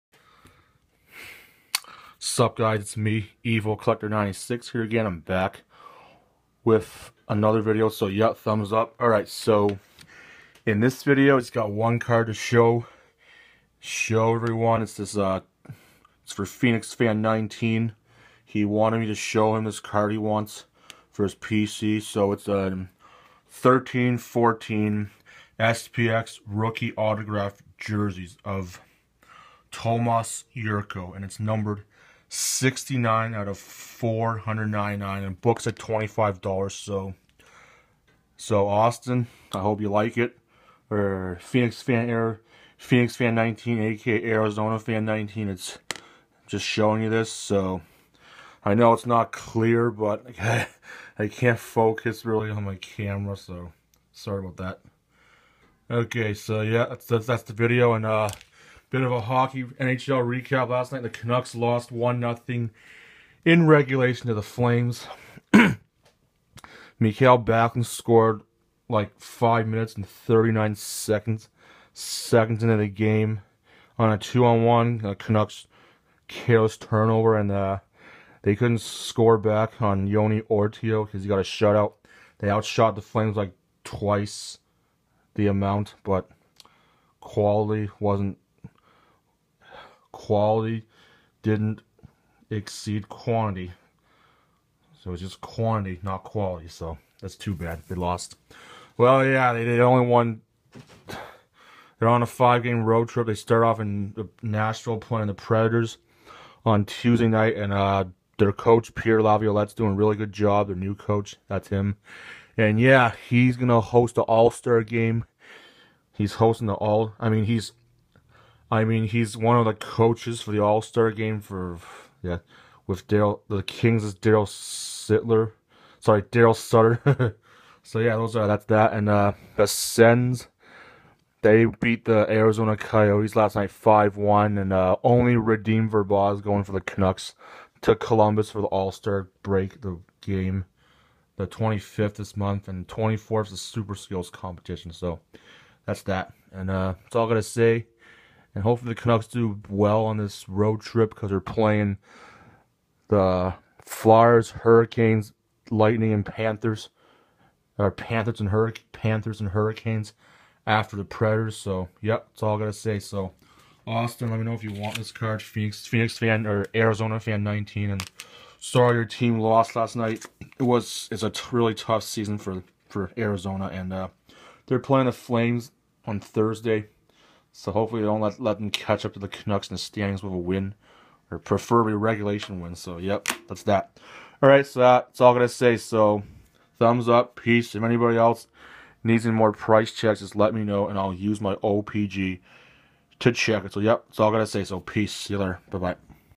Tomas Jurco card for phxfan19
It's a 13/14 SPx Rookie Autographed Jersey RC /499 Tomas Jurco.